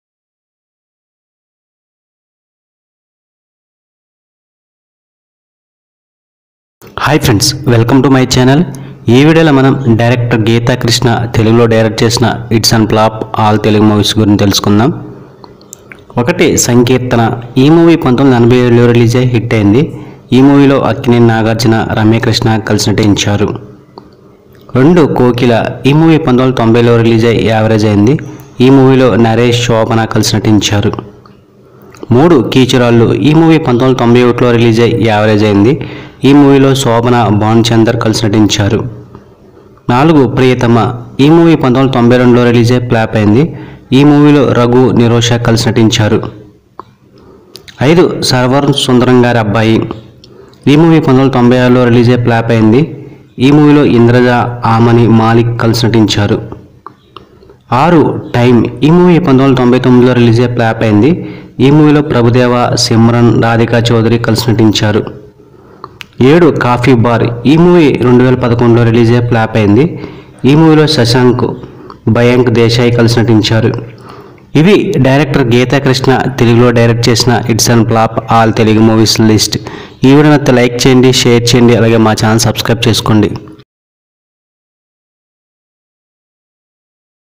Director Geetha Krishna Hits And Flops All Telugu Movies List
director geetha krishna movies
1.sankeerthana movie
2.keechurallu movie
3.kokila movie
4.priyatama movie
5.server sundaramgari abbaayi
6.time movie
7.coffee bar movie

telugu hits and flops
telugu all movies list
telugu movies
telugu movies list
telugu hits
telugu hit movies
telugu latest movies